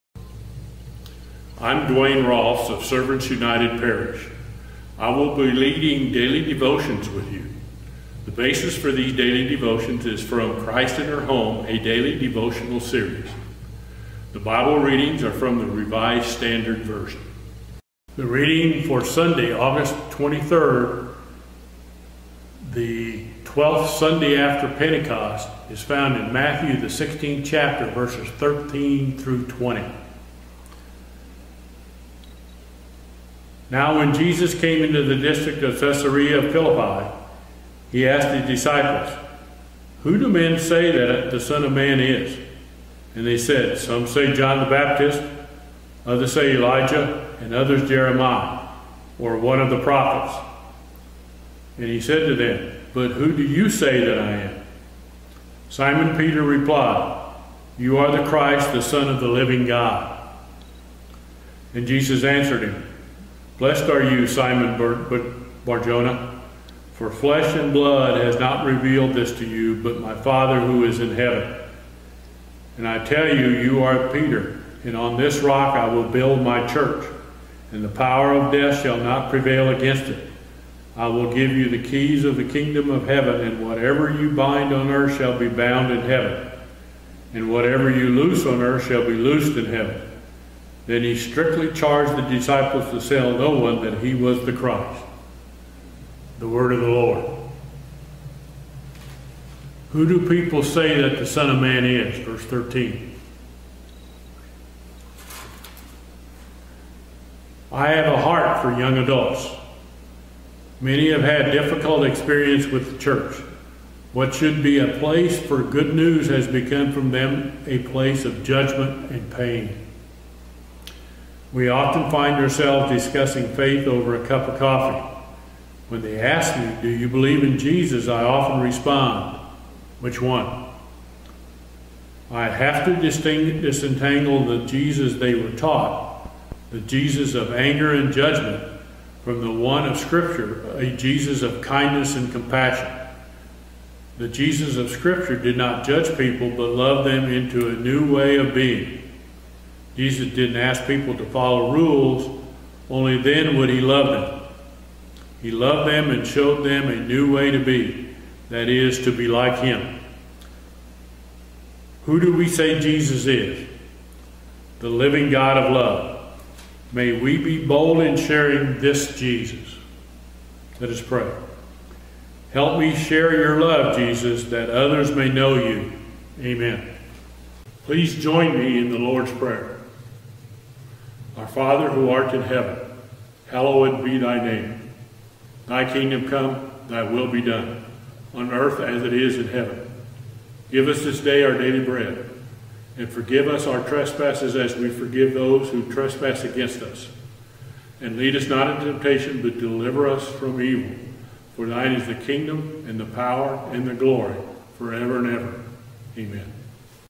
Our Daily Devotion August 23, 2020
Our Daily Devotion for August 23, 2020.  Reading by Duane Roelfs.